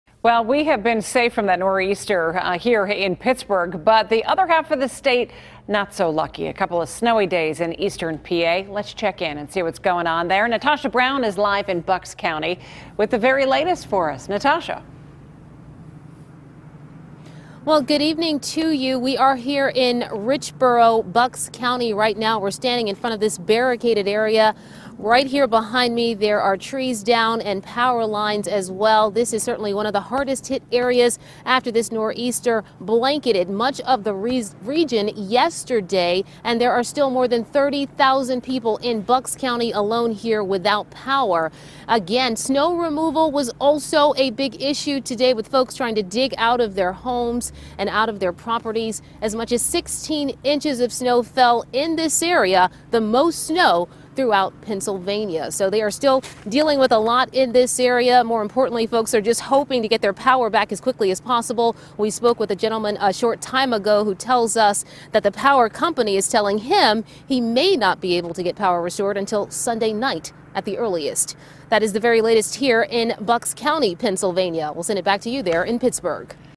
Storm Leaves Thousands Powerless In Pennsylvania
Pennsylvania utility companies say more than 110,000 customers are without power following the state's second major snowstorm in less than a week; CBS 3's Natasha Brown reports.